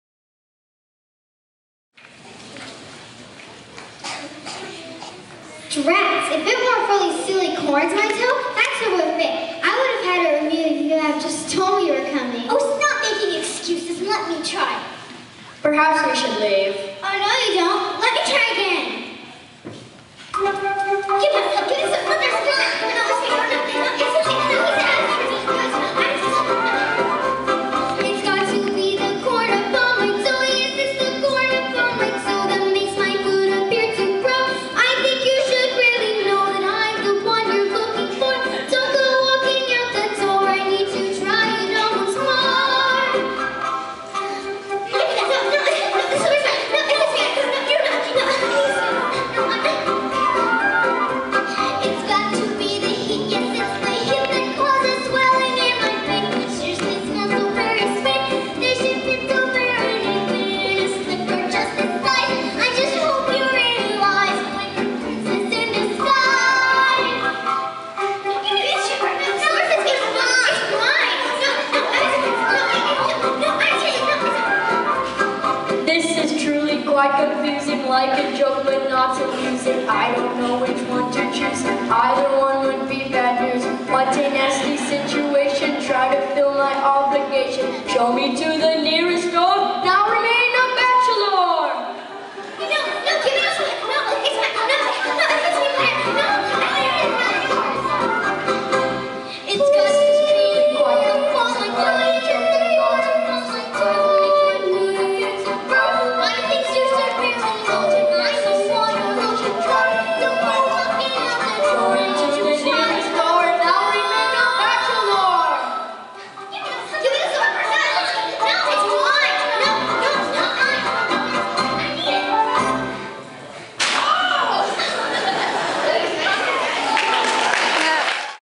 CoCo's Play
Courtneys play at school called the granny awards playing the evil stepsister